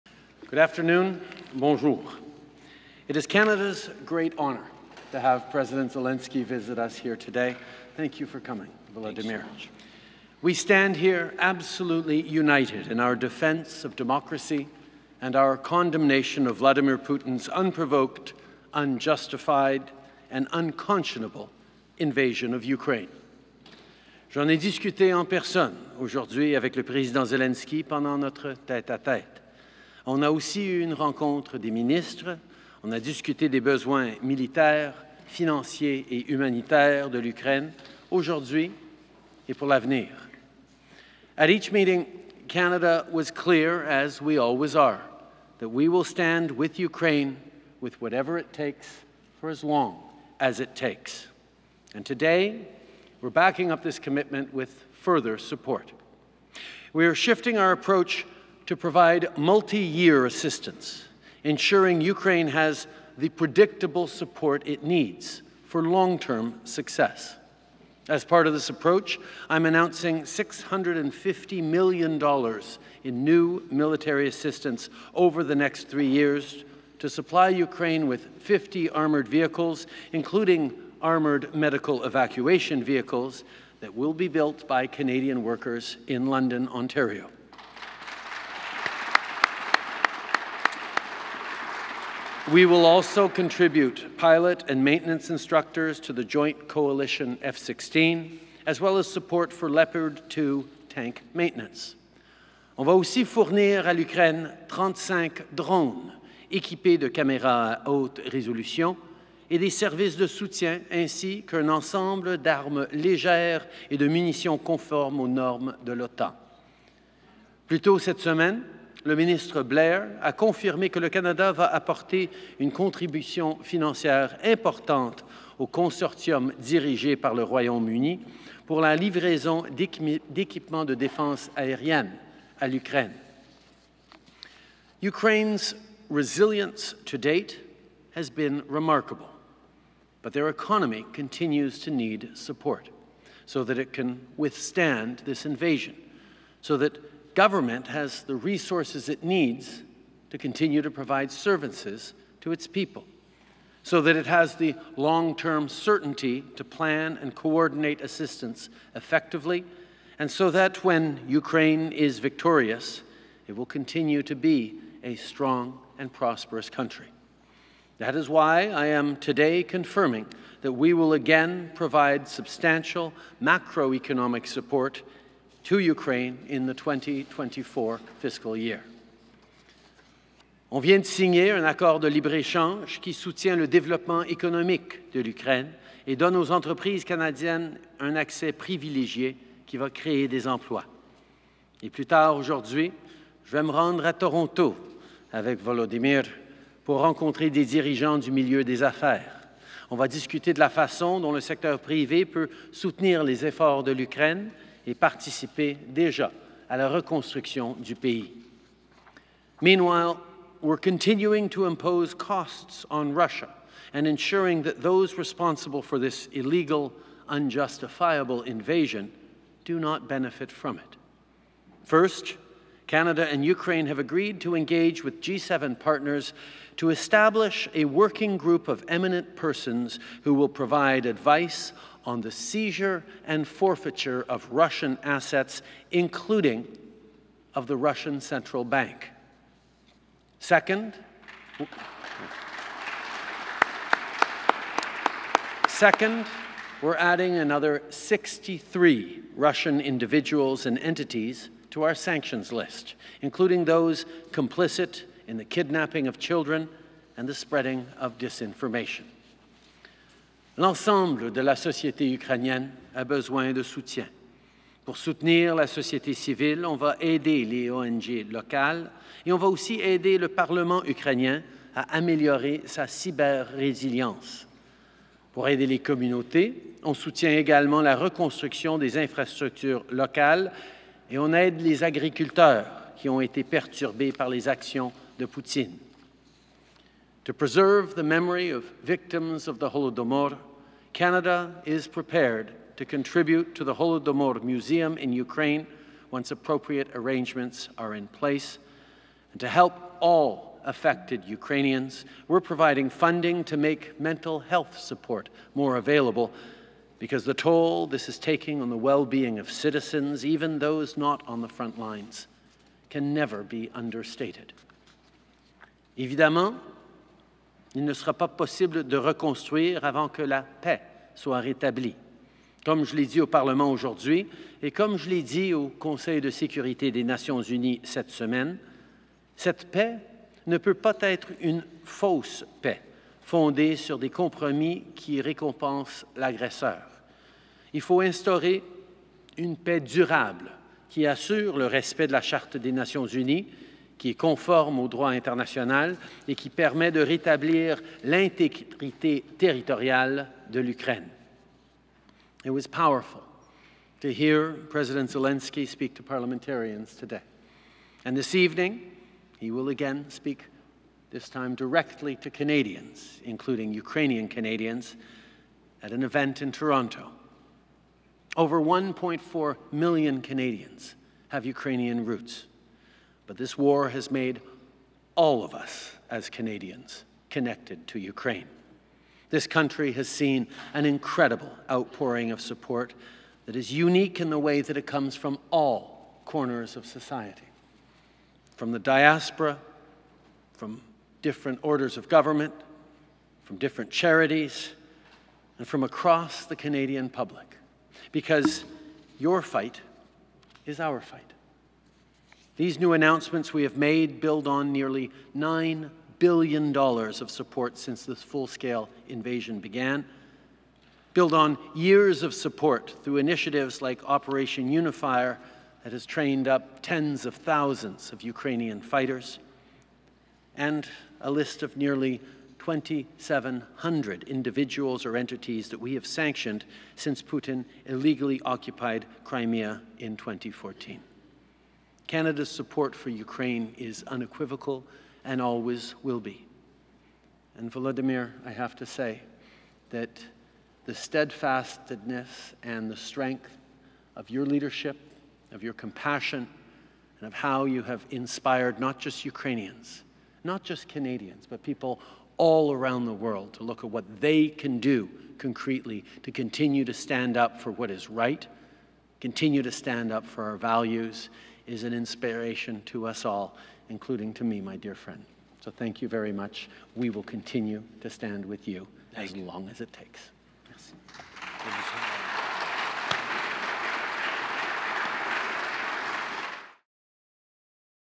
Le Canada réaffirme son soutien indéfectible à l’Ukraine aussi longtemps qu’il le faudra
22 septembre 2023
Le premier ministre Justin Trudeau annonce que le Canada fournira une assistance pluriannuelle à l’Ukraine et lui garantit le soutien prévisible dont elle a besoin pour réussir à long terme.